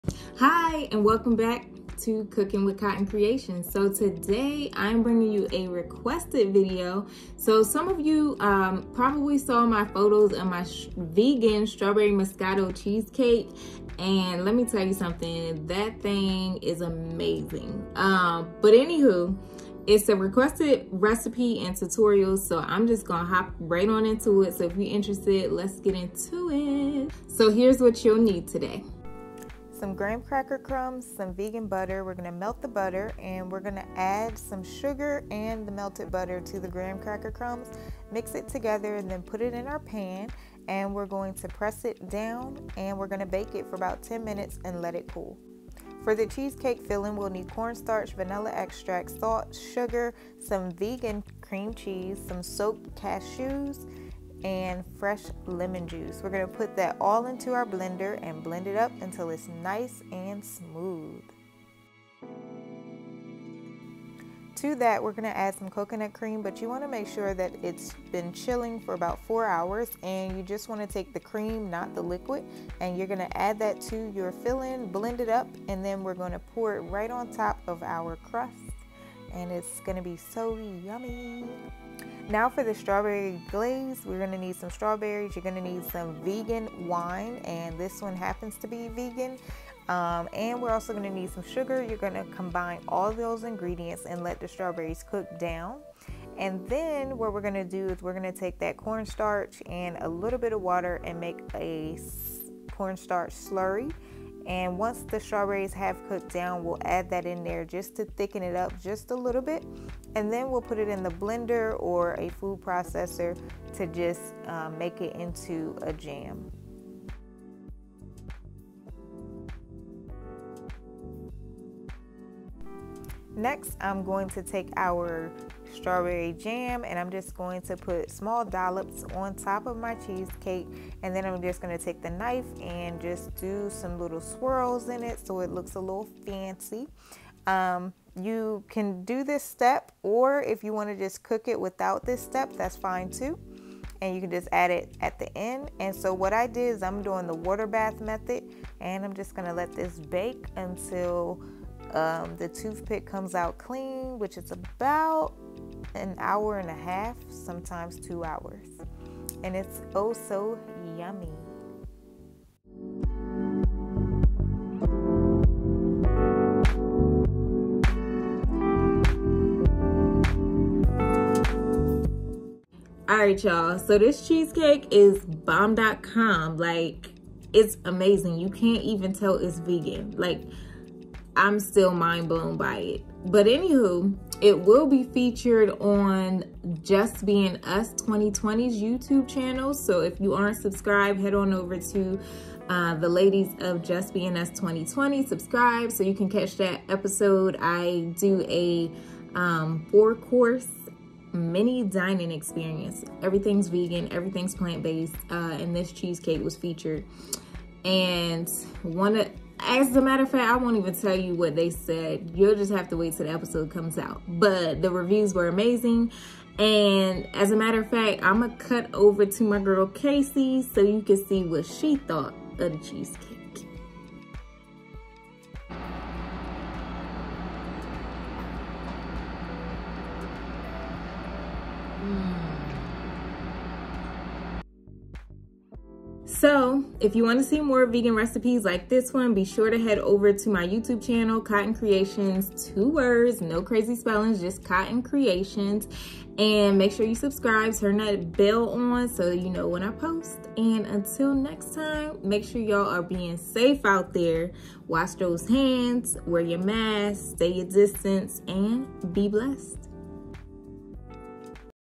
Strawberry Moscato Cheesecake
In this video, I share my Vegan Cheesecake recipe made from cashews. I top it with a strawberry Moscato glaze. Yum😋 and coconut whipped cream.

Follow on Instagram @CottonCreationsIG